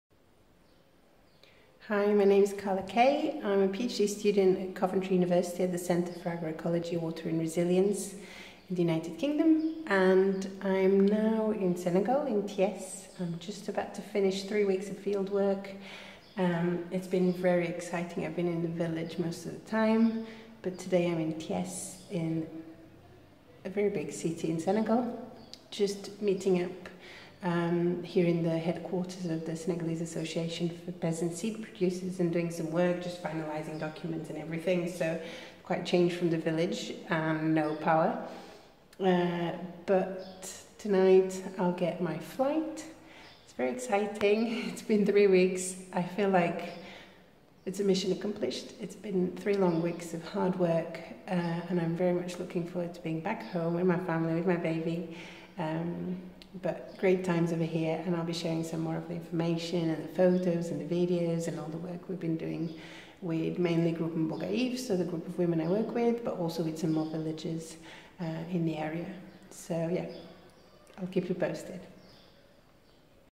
Time to head back home!
It's time to head back home. Here I am just finalising my work before heading back home from Senegal to the UK. It's a long trip but it will be rewarding, I'll get to see my family at the end!

For more information about my PhD, have a look at my website and blog